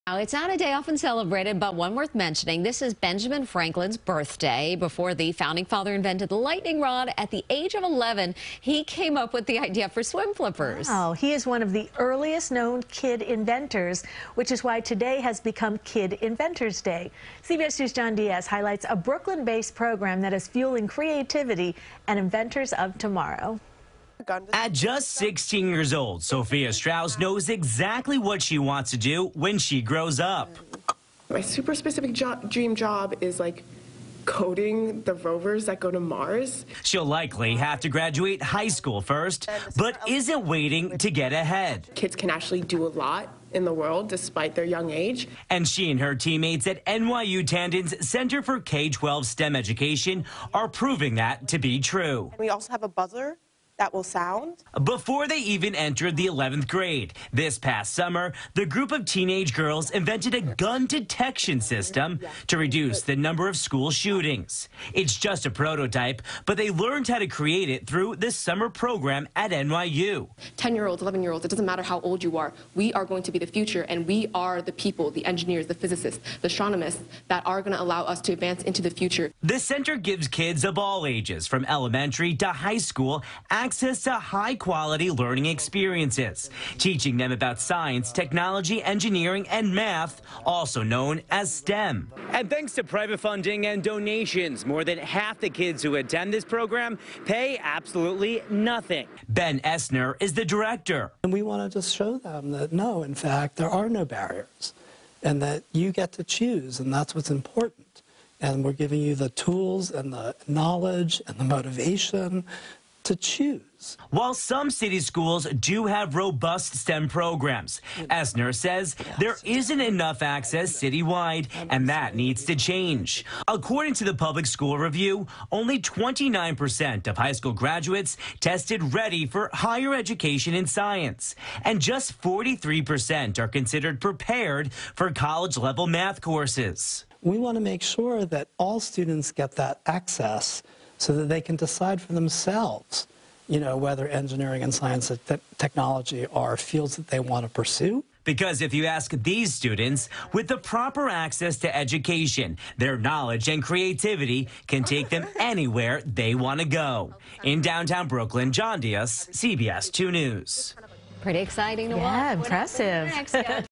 Benjamin Franklin's birthday inspires "Kid Inventors Day"
NYU Tandon School of Engineering's Center for K12 STEM Education is fueling creativity and inventors of tomorrow.  CBS2's John Dias has more.